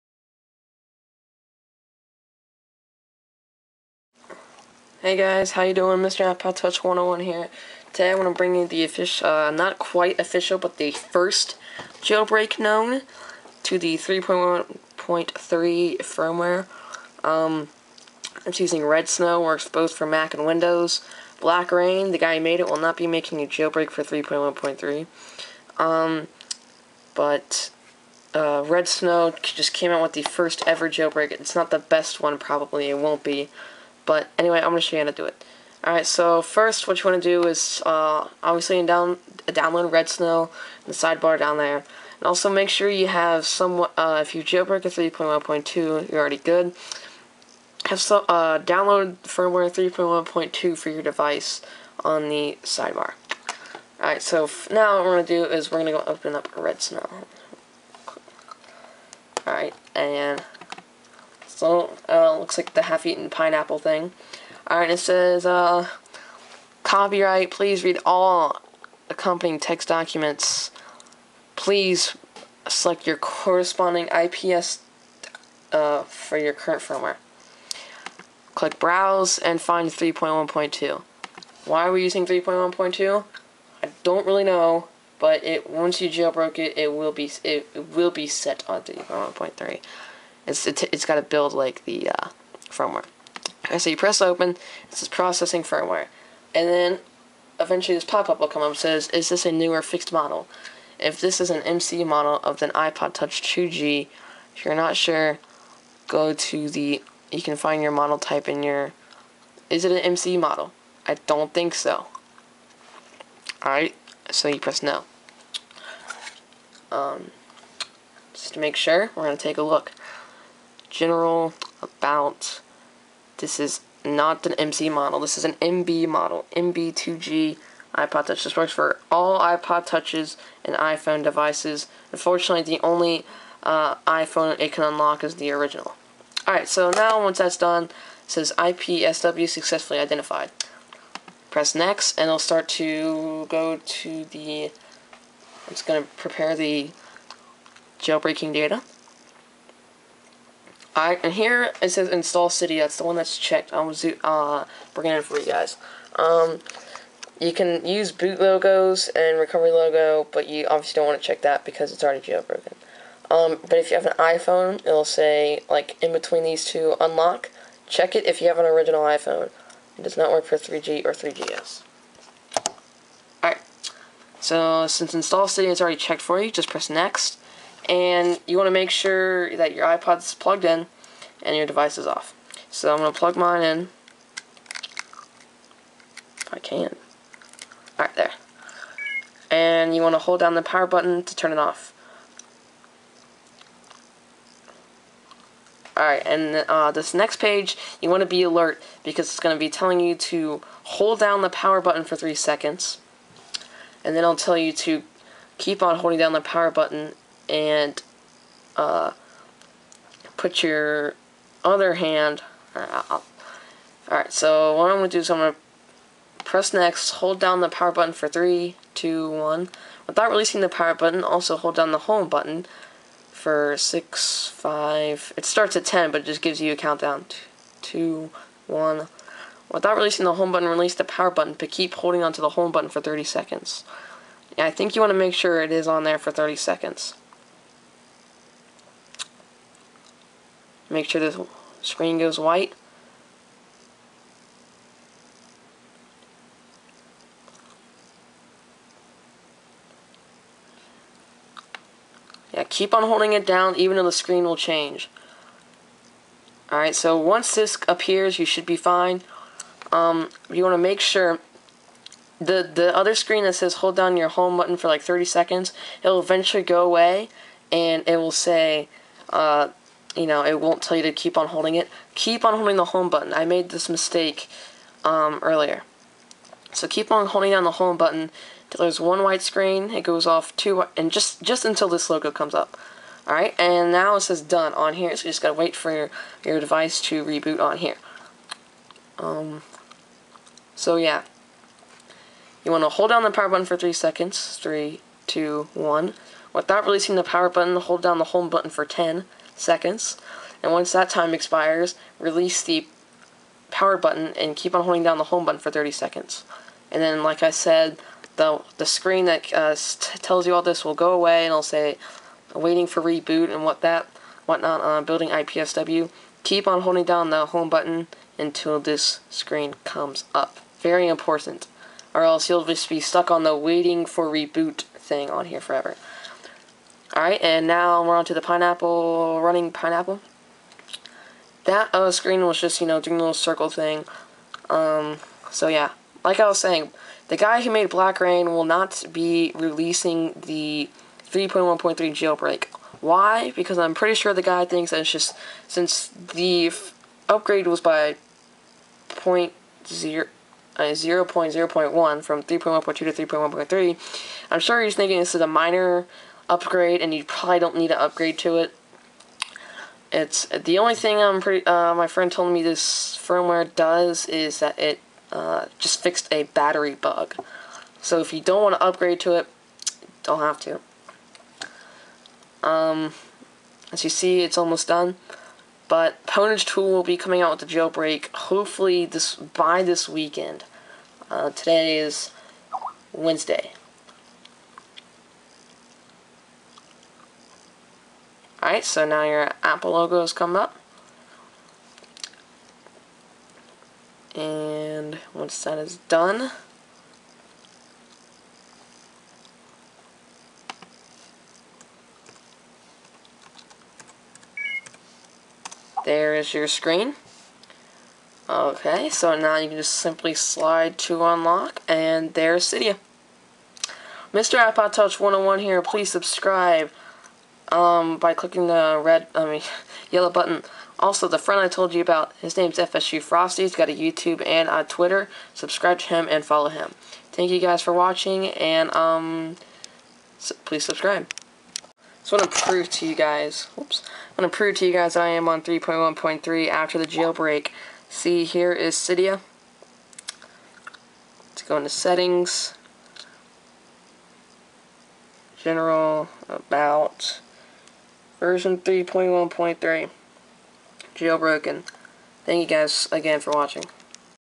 3.1.3 Jailbreak using Redsn0w (both for windows and mac)
works for : iPhone 2g 3g, ipod touch 1g and 2g NO MC model

scrol to find it